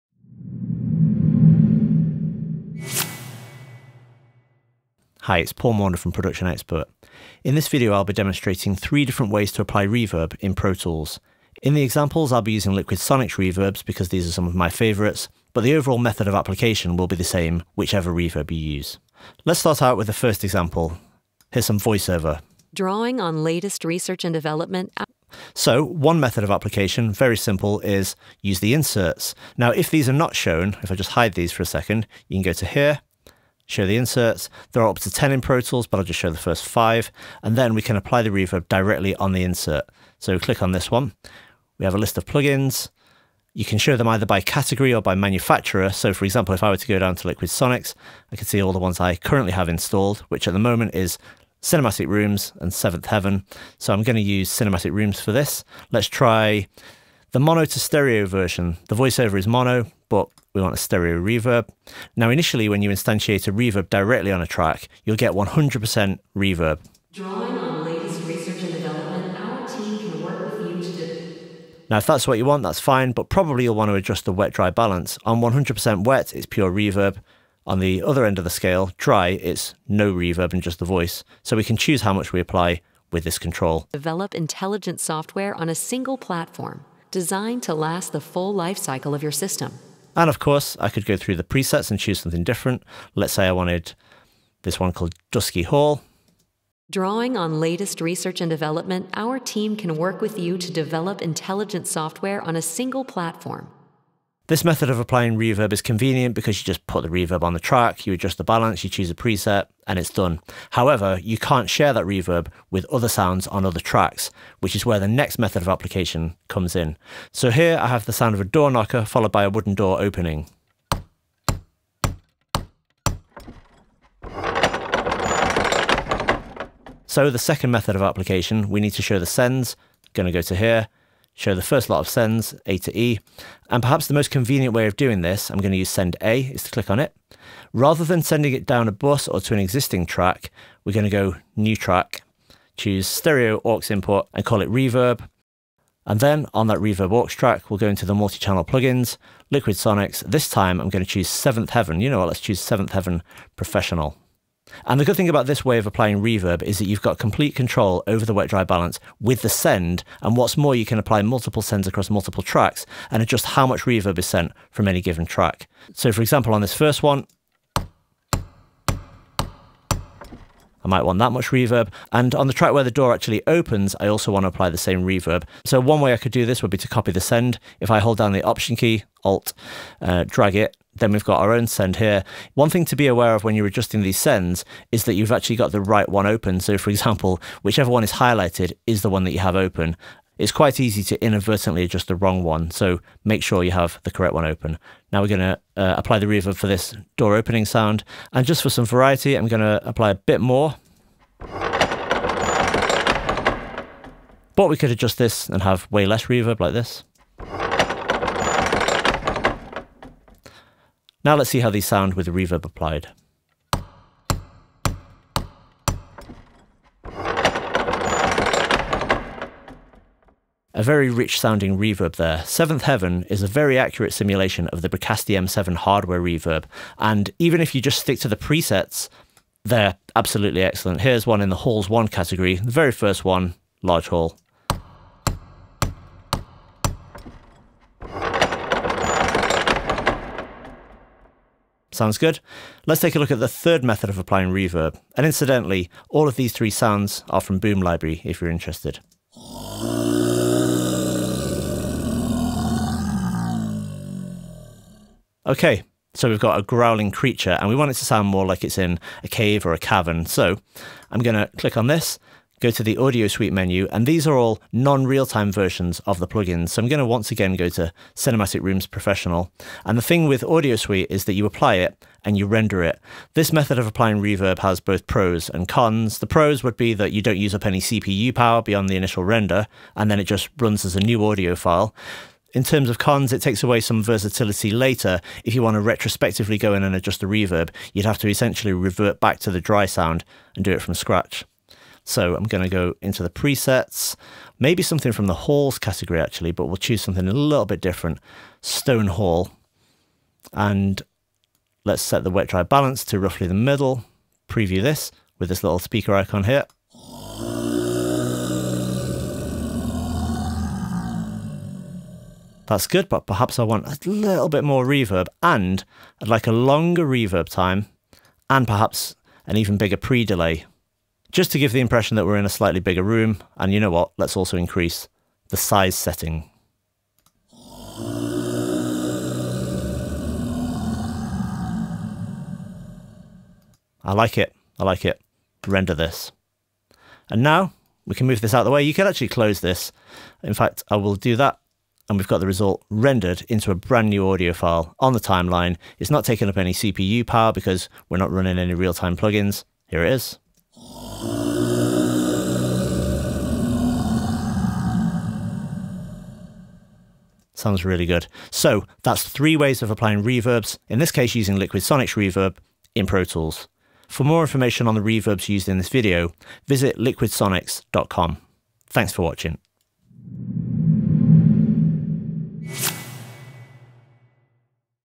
3 Ways To Apply Reverb In Pro Tools
In this video, Paul Maunder demonstrates 3 different ways to use reverbs in Pro Tools using LiquidSonics reverb plugins.

Head over to the Production Expert blog to read our full article…